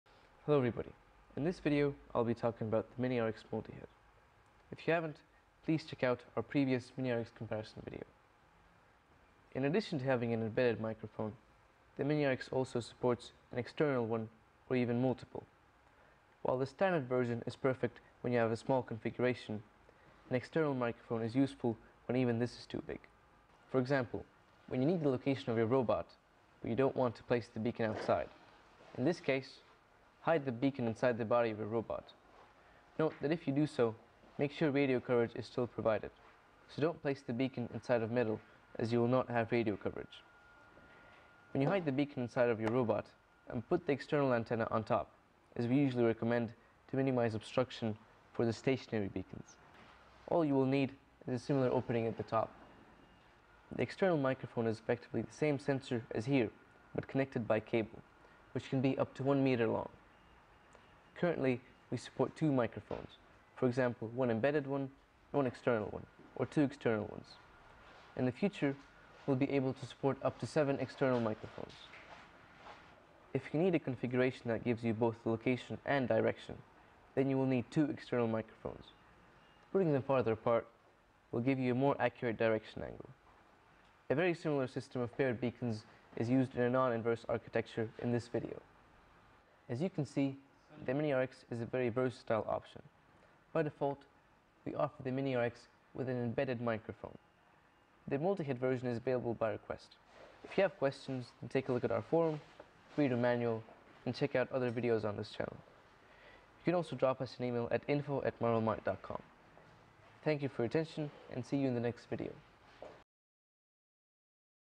Multi-head Mini-RX beacon
Comparison of different configurations of Mini-RX beacon:
- Standard
- Single additional head
- Two additional heads

The multi-head configurations are particularly great for:
- Simultaneous Location+Direction
- Hidden usage inside robots